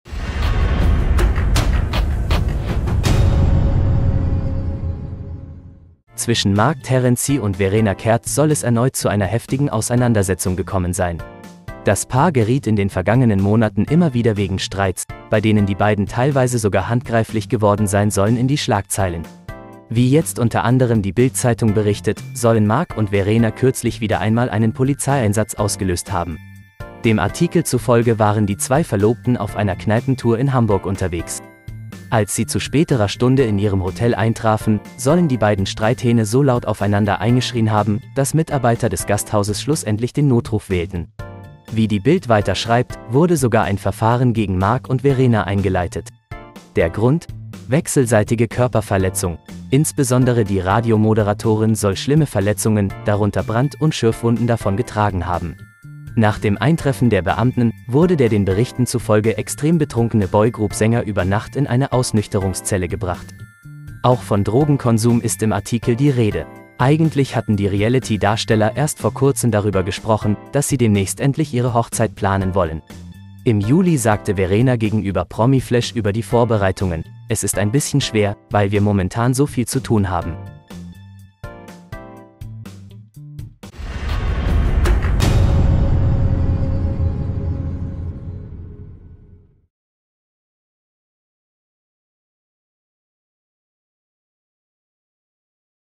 Marc Terenzi und Verena Kerth: Eskalierter Streit führt zu Polizeieinsatz und Verfahren!
In diesem Promi-News-Video erfährst du alles über den neuesten Zwischenfall zwischen Marc Terenzi und Verena Kerth. Es scheint, als ob sich ihr anhaltender Streit erneut zugespitzt hat. Wir werfen einen Blick auf die jüngsten Vorfälle, bei denen die beiden in die Schlagzeilen geraten sind.

In den letzten Monaten sorgten Marc Terenzi und Verena Kerth immer wieder für Schlagzeilen, weil ihre Auseinandersetzungen oft außer Kontrolle gerieten. Einigen Berichten zufolge kam es sogar zu körperlichen Auseinandersetzungen zwischen den beiden.

Wie kürzlich von der BILD berichtet wurde, führte ihr jüngster Streit zu einem Polizeieinsatz in Hamburg. Die beiden waren angeblich auf einer Kneipentour unterwegs und gerieten in ihrem Hotelzimmer in einen heftigen Streit, der so laut wurde, dass Mitarbeiter des Hotels den Notruf wählen mussten.

Wie die BILD weiter berichtet, wurde gegen Marc und Verena ein Verfahren wegen "wechselseitiger Körperverletzung" eingeleitet. Insbesondere Verena soll schwere Verletzungen erlitten haben, darunter Brand- und Schürfwunden.

Nach dem Vorfall wurde Marc Terenzi aufgrund seines extremen Alkoholkonsums in eine Ausnüchterungszelle gebracht. Es wird auch über Drogenkonsum in Verbindung mit diesem Vorfall berichtet.

Es ist bemerkenswert, dass Marc und Verena erst kürzlich ihre Hochzeitspläne angekündigt hatten. Doch der aktuelle Vorfall scheint diese Pläne vorerst auf Eis gelegt zu haben?! Wie Verena laut ‚Promiflash‘ im Juli sagte: "Es ist ein bisschen schwer, weil wir momentan so viel zu tun haben."

Verpasse nicht die neuesten Entwicklungen in diesem aufsehenerregenden Promi-Streit! Welche Meinung hast du zu diesem Vorfall? Teile sie in den Kommentaren!